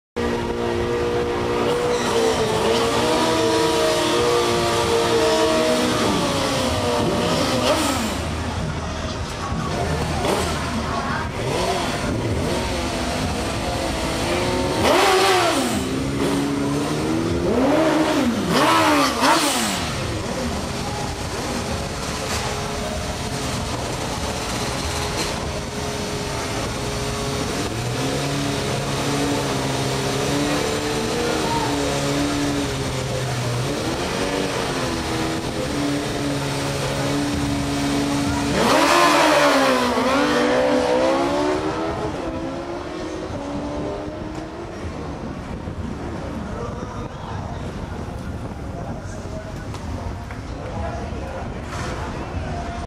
NATIONAL RECORD RUN GSX-R 750 PROJECT 10.07@137.79MPH STREET QUALIFYING 2ND ROUND
DANIELO POLISHING & PERFORMANCE#1s IN GSXR 750 PERFORMANCE IN P.R.,DRIVER ;RAFAEL ARROYO,FASTEST GSX-R 750 IN P.R. STOCK MOTOR ON PUMP GAS,STOCK WHEELBASE,FOOT SHIFTED,NATIONAL MOTORCYCLIST DAY APRIL5,2009